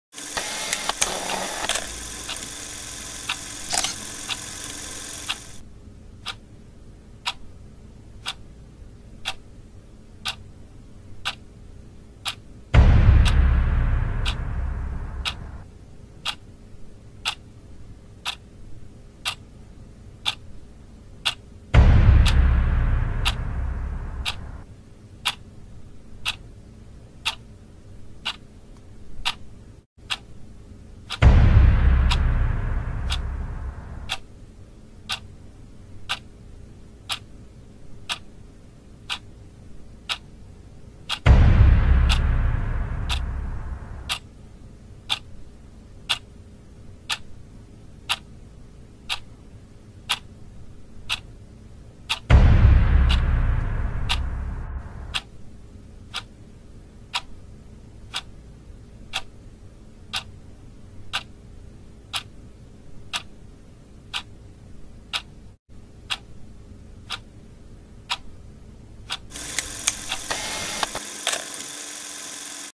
Cuenta atrás
El numero víctimas infantiles de la desnutrición causada por el acaparamiento de tierras alcanza unas cifras tan astronómicas que parece imposible hacernos una representación mental clara de la tragedia. Con este vídeo pretendemos exponerlo con una metáfora visual y sonora.